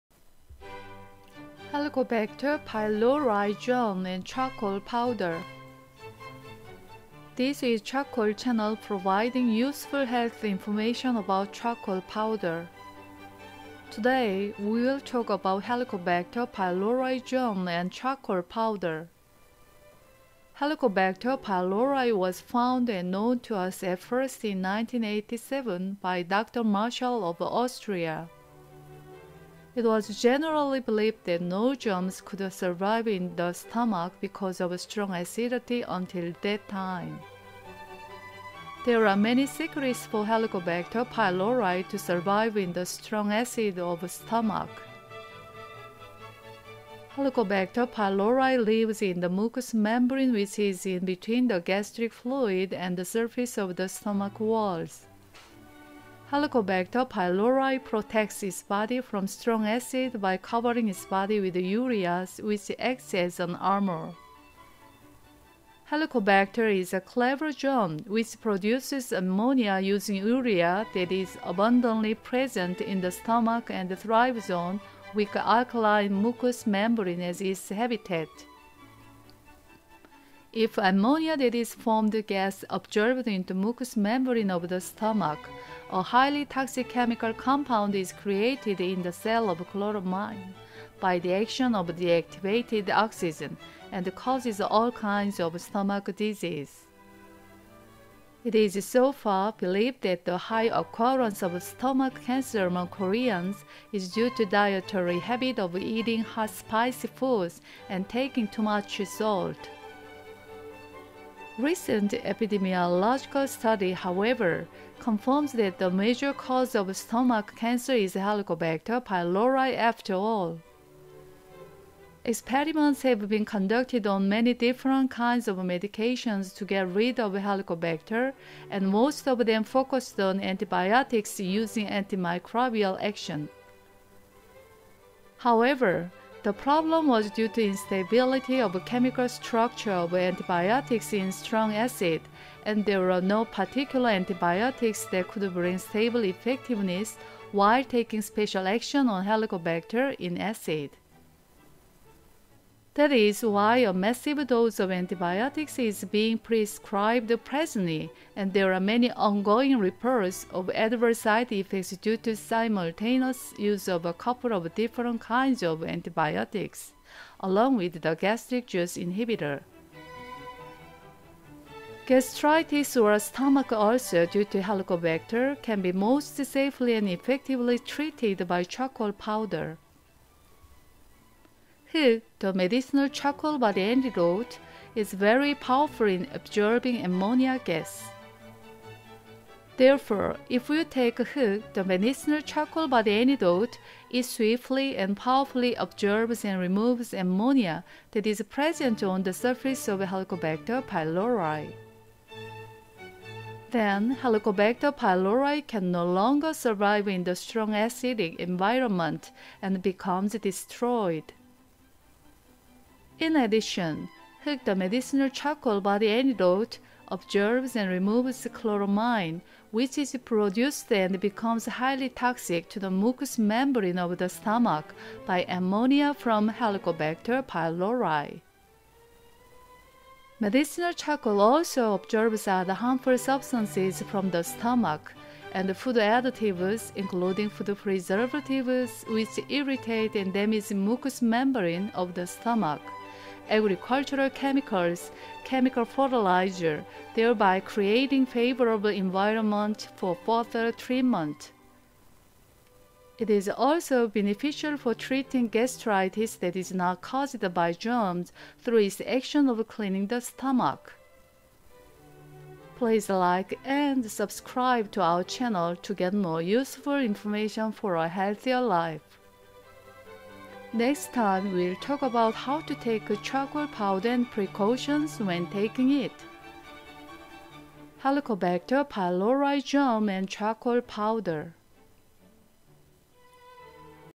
Helicobacter Pylori Germ and Charcoal Powder, the effect of medicinal charcoal powder 57
Gastritis or stomach ulcer due to Helicobacter can be most safely and effectively treated by charcoal powder. If you take 「Heuk」, the medicinal charcoal body antidote, it swiftly and powerfully absorbs and removes ammonia that is present on the surface of Helicobacter pylori.